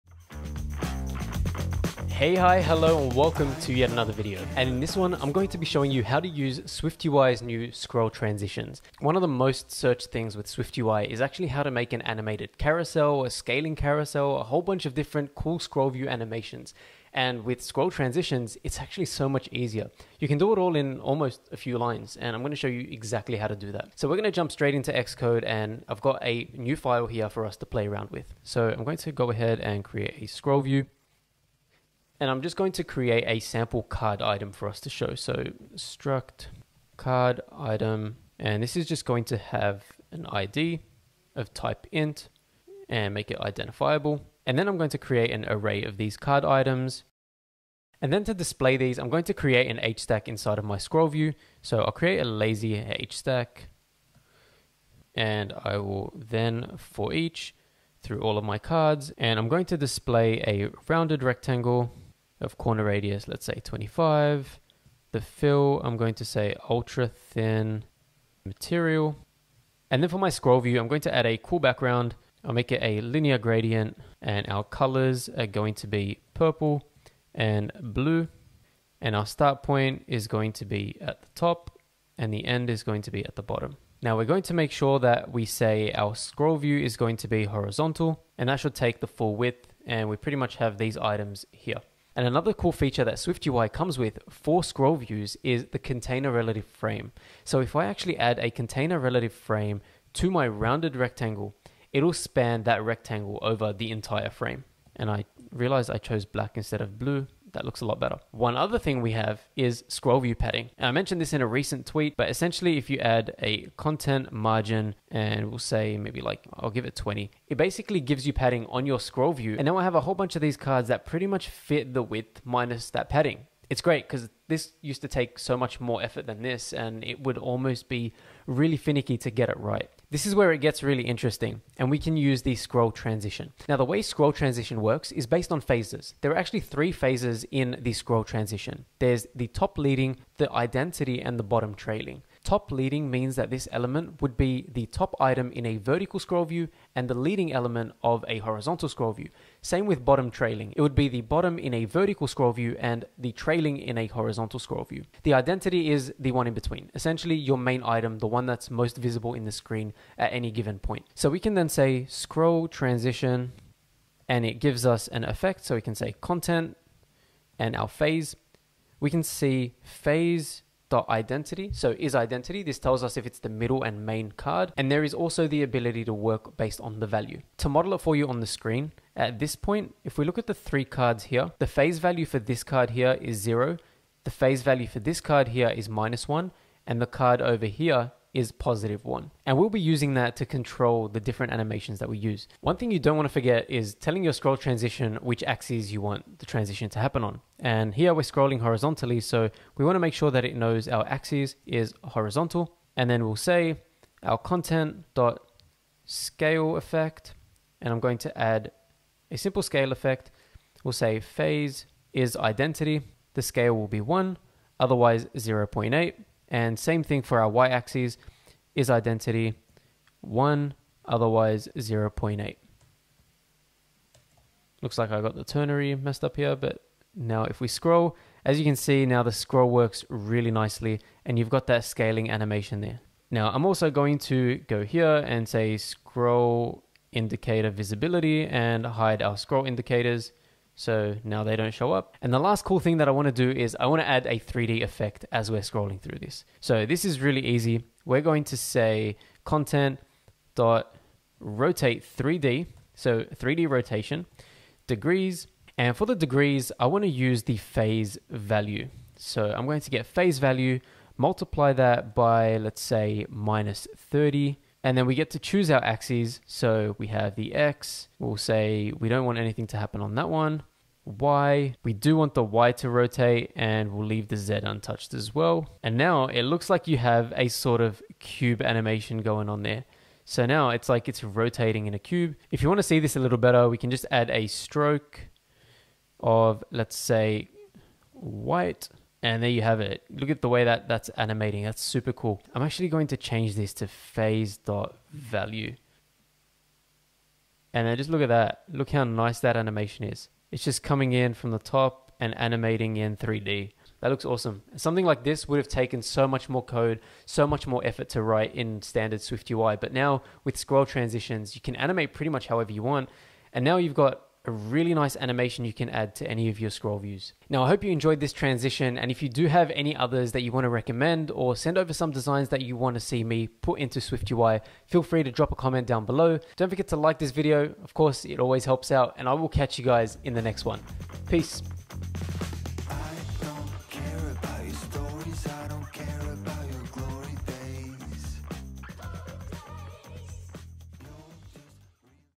ScrollView Animations in SwiftUI | iOS17 | WWDC23
Hey, hi, hello!
In this video, we'll be exploring the new Scroll Transition's modifier in SwiftUI. Now you can build sophisticated, classy, and aesthetically pleasing scroll carousels and animations with just a few lines of code.

This will help you boost your user experience and create stunning UIs. And the best part? It's super easy!

Stay curious and keep coding!